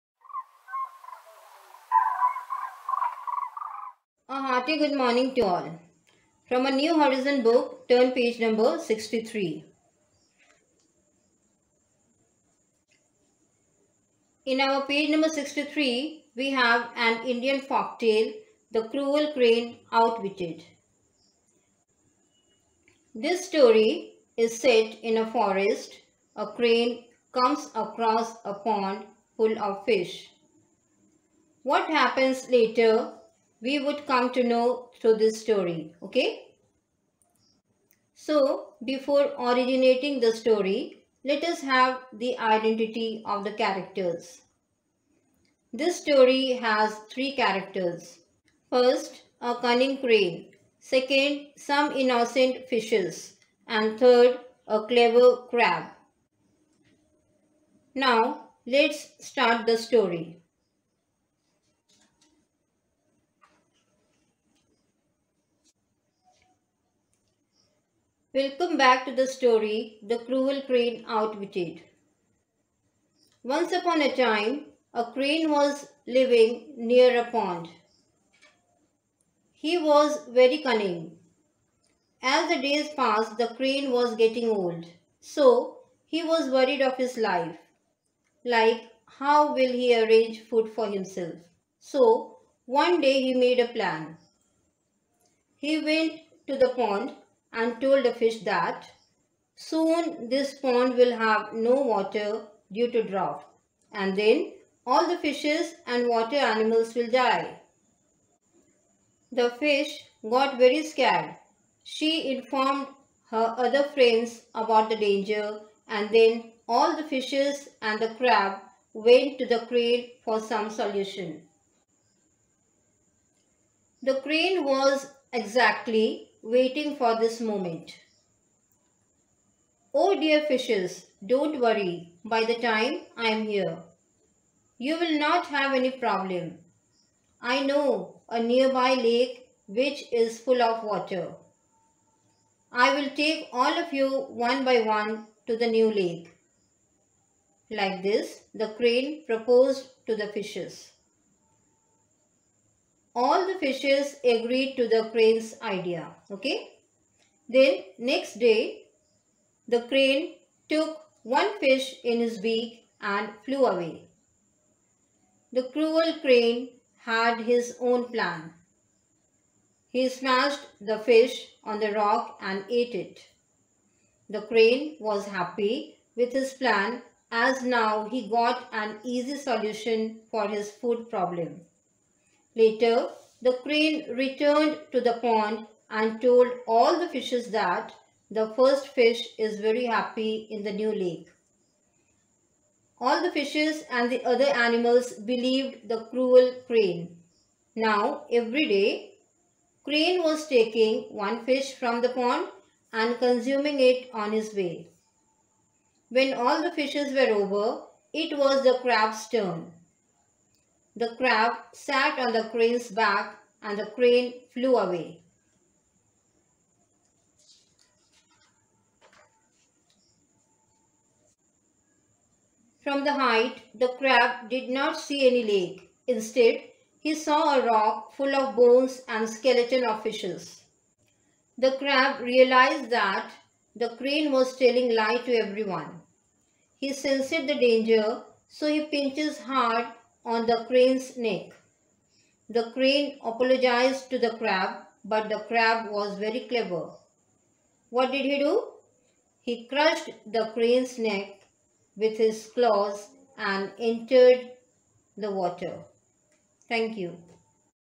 Class 4 English Literature Lesson 11 (The Cruel Crane Outwitted)
Story name: The Cruel Crane Outwitted

Don't think evil of anyone.